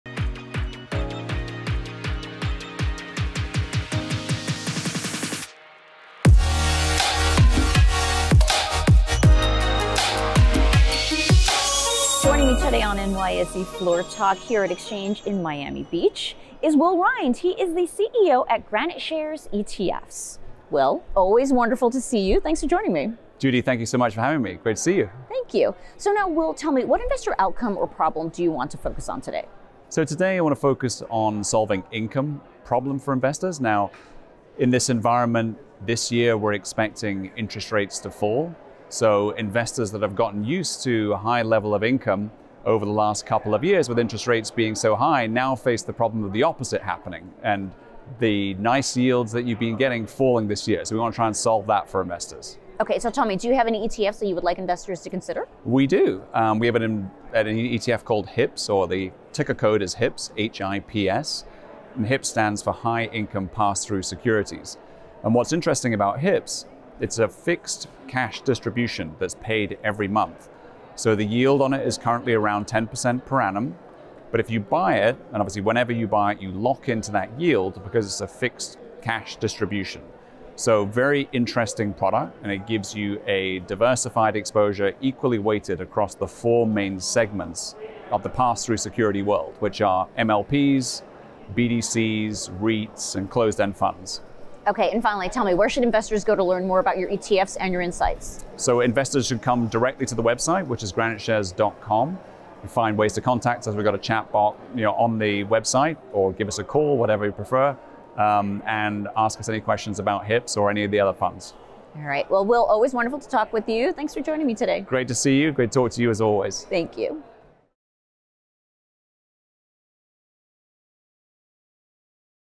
NYSE TV’s Floor Talk: Will Rhind of GraniteShares ETFs
NYSE’s Judy Shaw sits down with Will Rhind of GraniteShares ETFs for Floor Talk at Exchange: An ETF Experience 2024